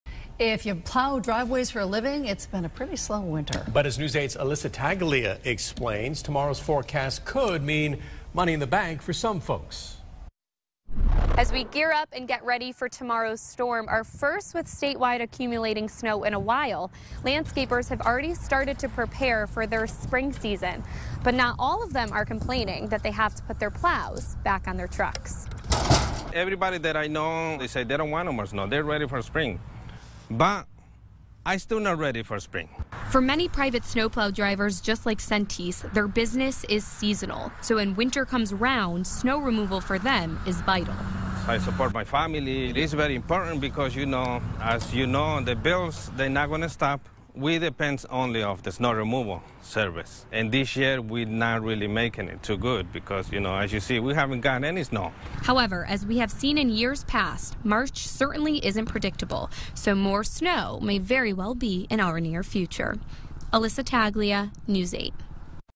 Plow crews gear up for Wednesday storm
As Connecticut gears up and gets ready for Wednesday's storm, our first storm with state wide snowfall accumulation in a while, landscapers have already started to prepare for their spring season. But not all of them are complaining that they have to put their plows back on their trucks.